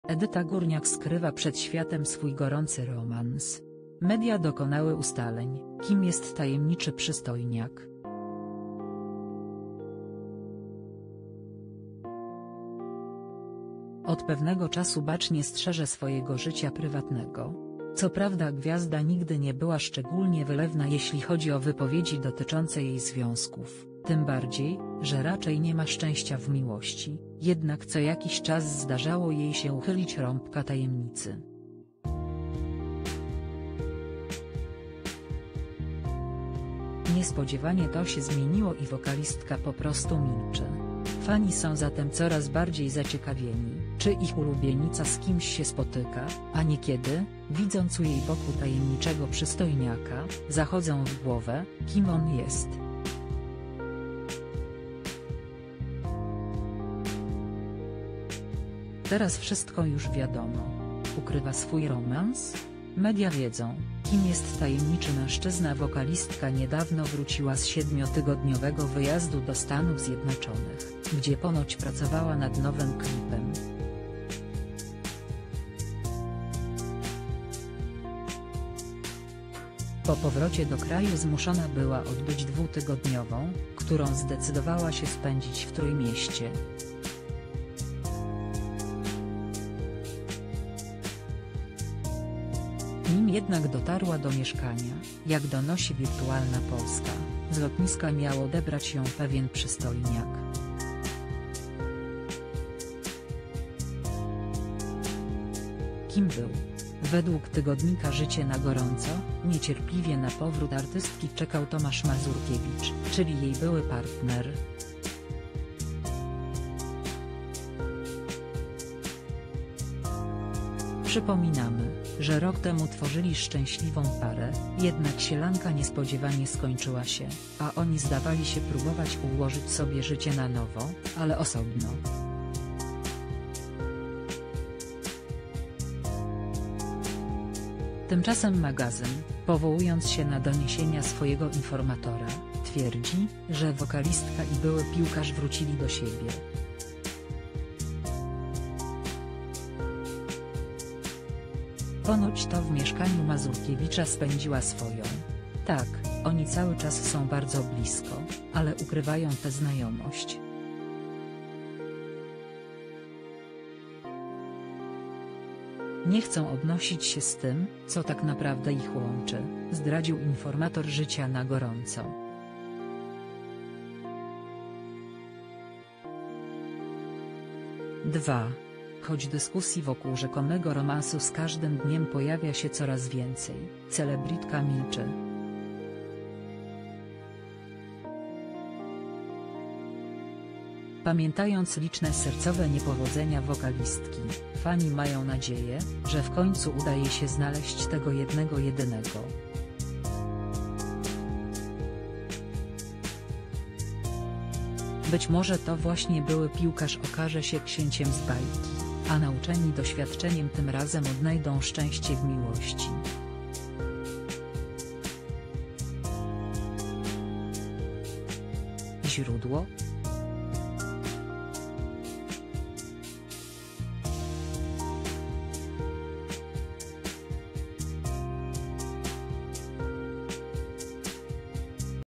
Gorący romans podczas kwarantanny? Media dotarły do ustaleń, kim jest przystojniak, który zawrócił w
Edyta Górniak skrywa przed światem swój gorący romans. Media dokonały ustaleń, kim jest tajemniczy przystojniak.    od pewnego czasu bacznie strzeże swojego życia prywatnego. Co prawda gwiazda nigdy nie była szczególnie wylewna jeśli chodzi o wypowiedzi dotyczące jej związków, tym bardziej, że raczej nie ma szczęścia w miłości, jednak co jakiś czas zdarzało jej się uchylić rąbka tajemnicy. Niespodziewanie to się zmieniło i wokalistka po prostu milczy. Fani są zatem coraz bardziej zaciekawieni, czy ich ulubienica z kimś się spotyka, a niekiedy, widząc u jej boku tajemniczego przystojniaka, zachodzą w głowę, kim on jest. Teraz wszystko już wiadomo.    ukrywa swój romans? Media wiedzą, kim jest tajemniczy mężczyzna    Wokalistka niedawno wróciła z 7-tygodniowego wyjazdu do Stanów Zjednoczonych, gdzie ponoć pracowała nad nowym klipem. Po powrocie do kraju zmuszona była odbyć 2-tygodniową , którą zdecydowała się spędzić w Trójmieście. Nim jednak dotarła do mieszkania, jak donosi Wirtualna Polska, z lotniska miał odebrać ją pewien przystojniak. Kim był? Według tygodnika "Życie na gorąco", niecierpliwie na powrót artystki czekał Tomasz Mazurkiewicz, czyli jej były partner. Przypominamy, że rok temu tworzyli szczęśliwą parę, jednak sielanka niespodziewanie skończyła się, a oni zdawali się próbować ułożyć sobie życie na nowo, ale osobno.        Tymczasem magazyn, powołując się na doniesienia swojego informatora, twierdzi, że wokalistka i były piłkarz wrócili do siebie. Ponoć to w mieszkaniu Mazurkiewicza  spędziła swoją .    - Tak, oni cały czas są bardzo blisko, ale ukrywają tę znajomość. Nie chcą obnosić się z tym, co tak naprawdę ich łączy - zdradził informator "Życia na gorąco".        2.    Choć dyskusji wokół rzekomego romansu z każdym dniem pojawia się coraz więcej, celebrytka milczy. Pamiętając liczne sercowe niepowodzenia wokalistki, fani mają nadzieję, że w końcu uda jej się znaleźć tego jednego jedynego. Być może to właśnie były piłkarz okaże się "księciem z bajki", a nauczeni doświadczeniem tym razem odnajdą szczęście w miłości.      Źródło: